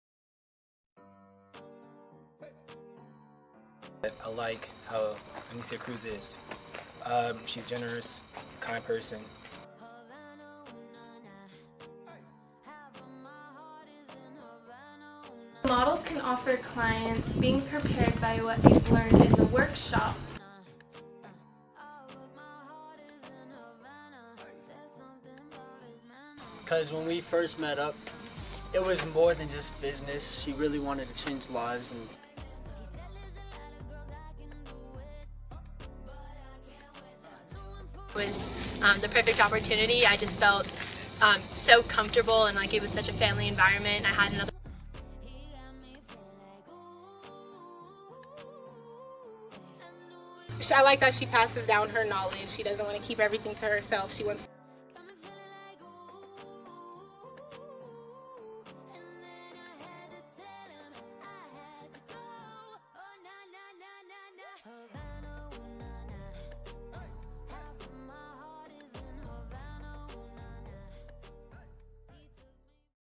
Natori's Club Introduction
Natori's Club presents a team of certified models, working together in the hot sun to produce amazing work as always.

***DO NOT OWN ANY COPYRIGHTS TO MUSIC***